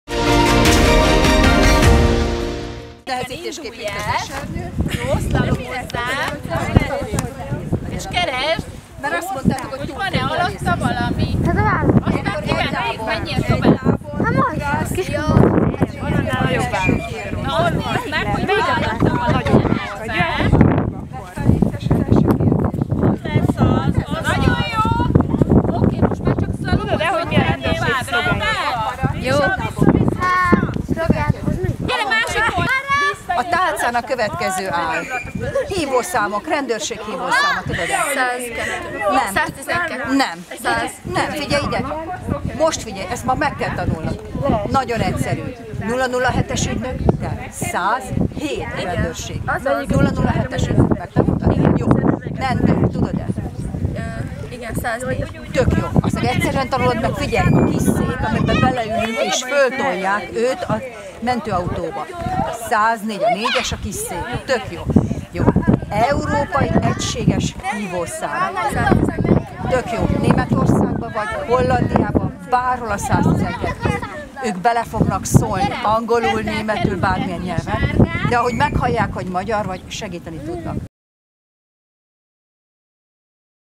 Bűnmegelőzési program ÁGOTA Falván
Bűnmegelőzési programot tartott csütörtökön „Biztonság napja 2024. tavasz” címmel a gyermekvédelmi gondoskodásban élő fiatalok számára az ÁGOTA Közösség a Bács-Kiskun Vármegyei Rendőr-főkapitánysággal karöltve ÁGOTA Falván. A rendezvényen öt vármegyéből mintegy száznegyven gyermekvédelmi gondoskodásban élő fiatal vett részt. Az esemény illeszkedett a Bács-Kiskun Vármegyei Rendőr-főkapitányság, a Szent Ágota Gyermekvédelmi Szolgáltató, valamint az ÁGOTA Alapítvány között 2023-ban megszületett együttműködési megállapodáshoz. Részletek